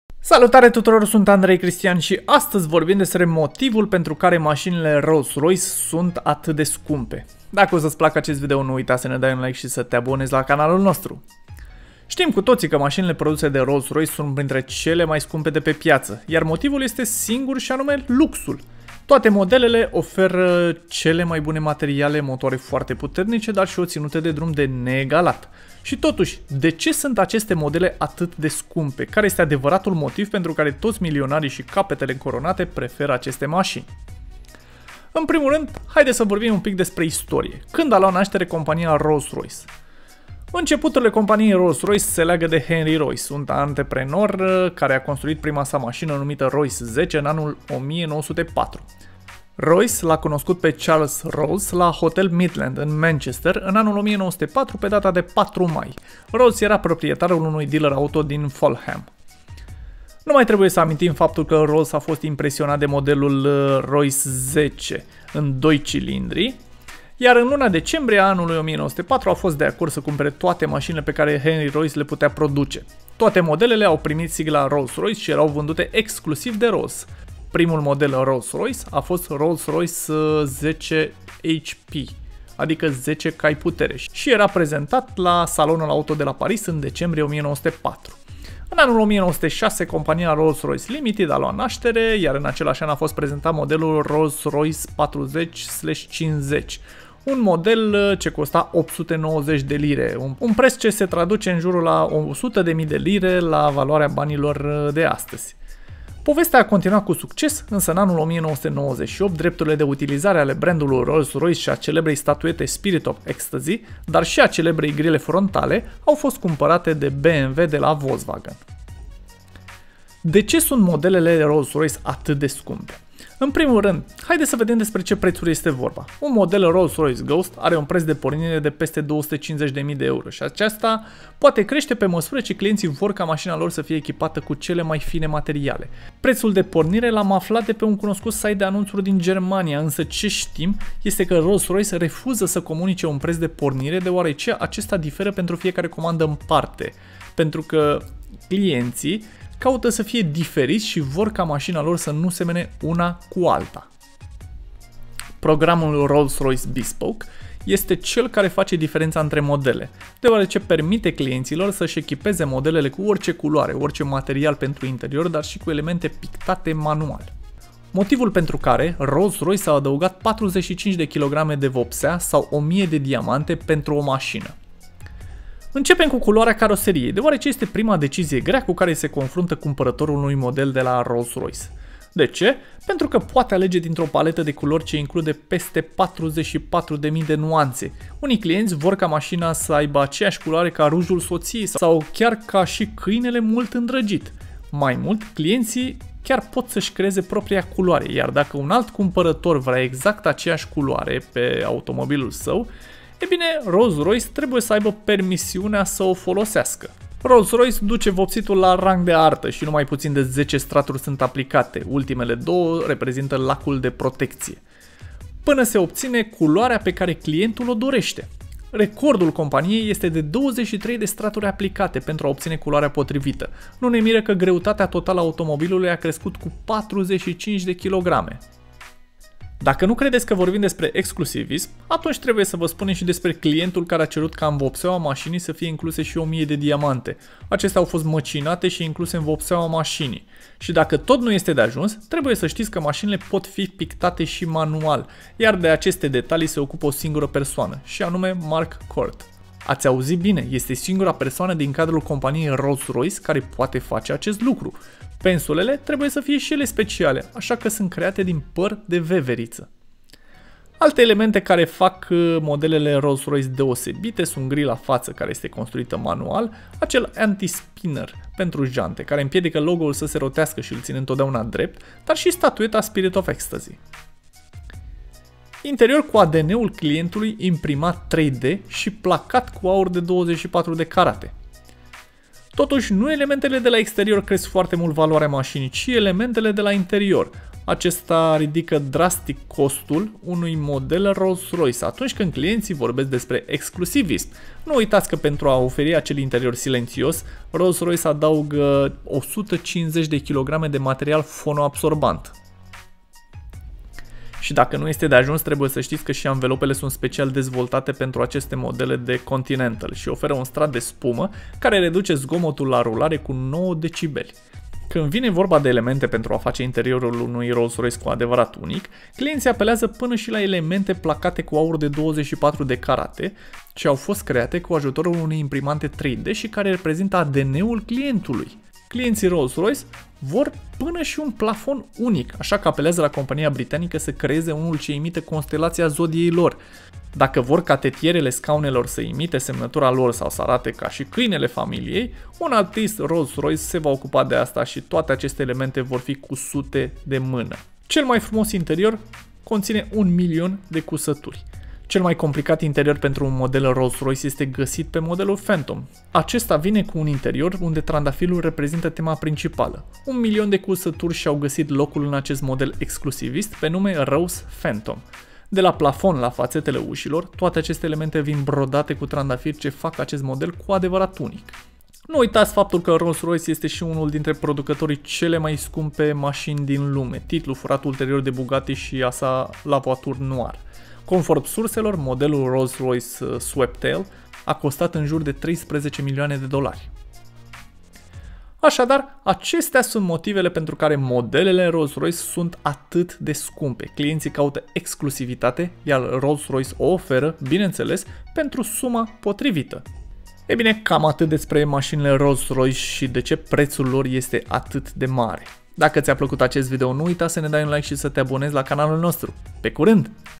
DE CE SUNT MODELELE ROLLS-ROYCE ATAT DE SCUMPE?
Stim cu totii ca masinile produse de Rolls-Royce sunt printre cele mai scumpe de pe piata, iar motivul este unul singur si anume luxul. Toate modelele ofera cele mai bune materiale, motoare foarte puternice, dar si o tinuta de drum de neegalat.

Si totusi, de ce sunt aceste modele atat de scumpe? Care este adevaratul motiv pentru care toti milionarii si capatele incoronate prefera aceste masini.